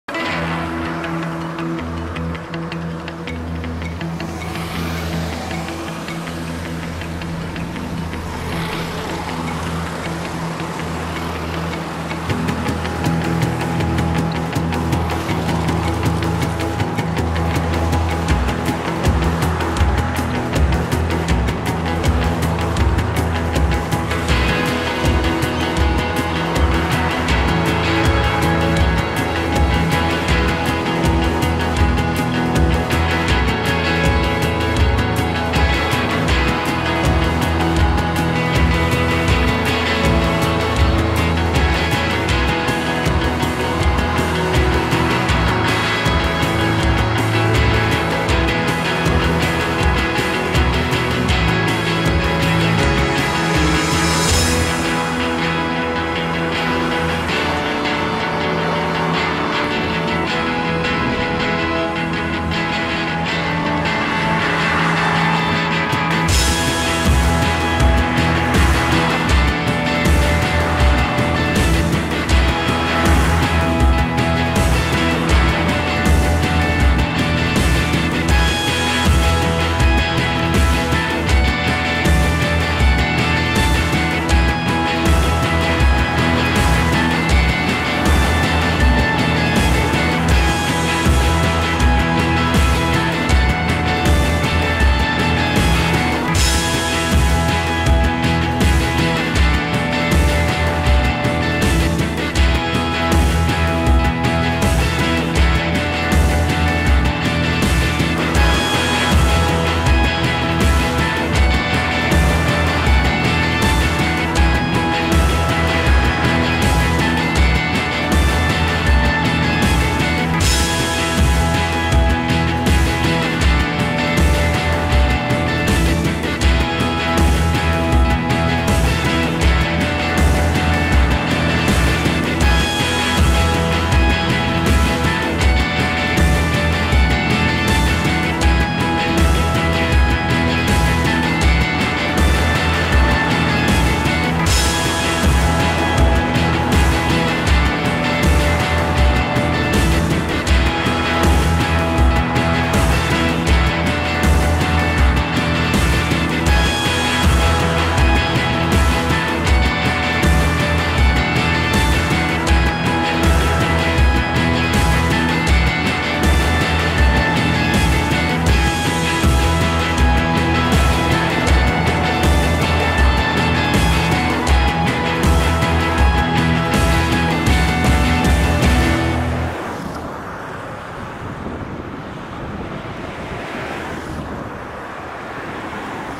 2020/06/19 大雨後の琵琶湖!! 風は弱くて穏やかだけど湖上はガラ空きです
2020年6月19日 梅雨入り二発目のまとまった雨で水位が10cm以上急上昇、全面泥濁りになった琵琶湖。雨が小止みになって風も弱く湖面は穏やかですが、さすがに平日でこの天気だとボートは少なくてガラ空きです。これはもしかしたらチャンスかも･･･ w（6月19日13時頃撮影）